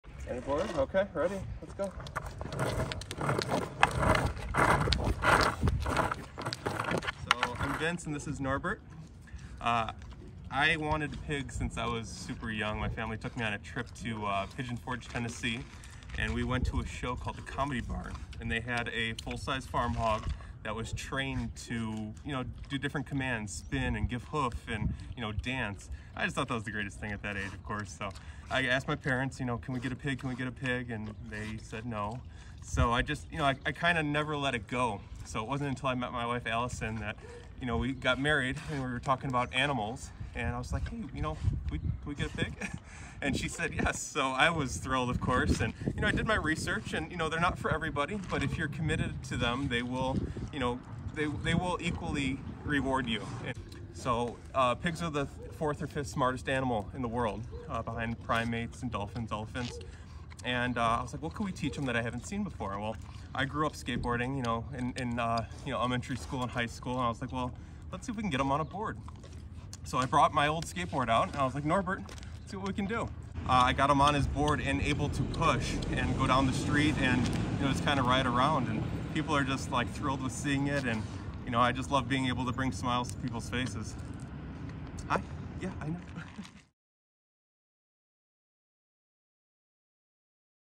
Norbert the pig rides a skateboard
Buffalo Grove residents Alicen and Vincent Baran have taught their pet pig, Norbert, to ride a skateboard.